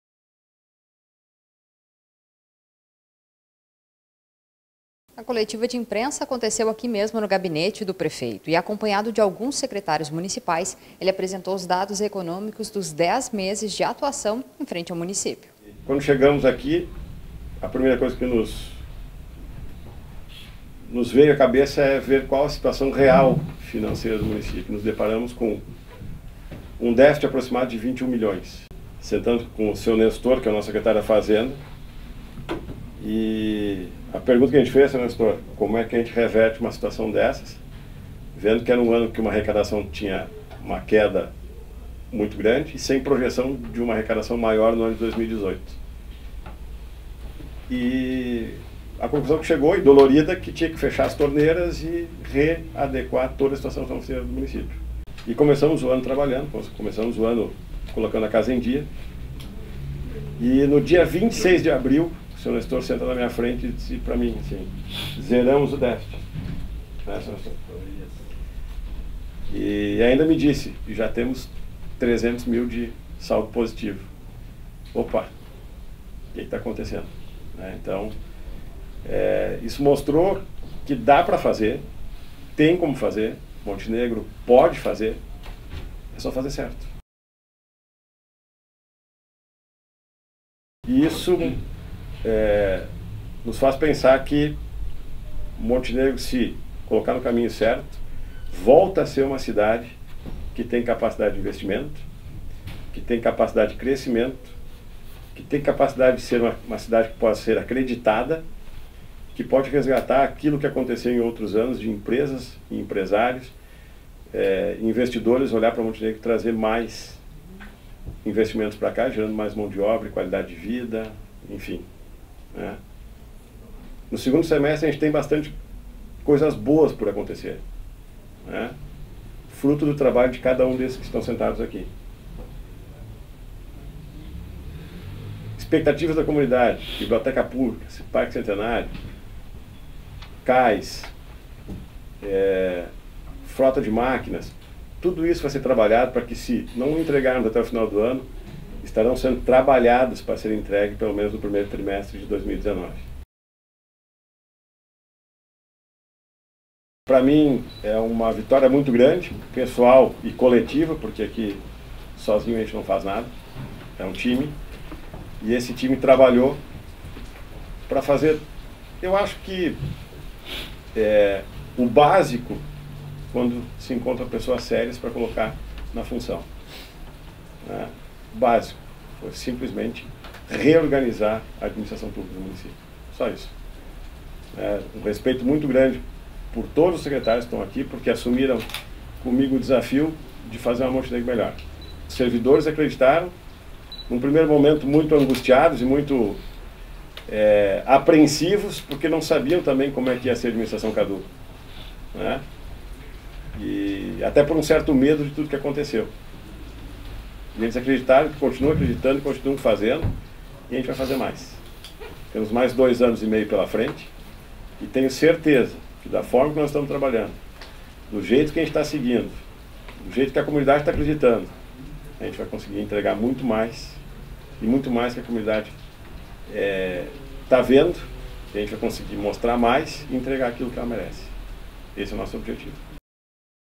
Prefeitura: Coletiva sobre 10 meses de governo
Kadu Müller completa 10 meses no comando de Montenegro. Na sexta-feira, em uma coletiva de imprensa, ele apresentou os índices desta gestão e destacou o trabalho desenvolvido pelos secretários.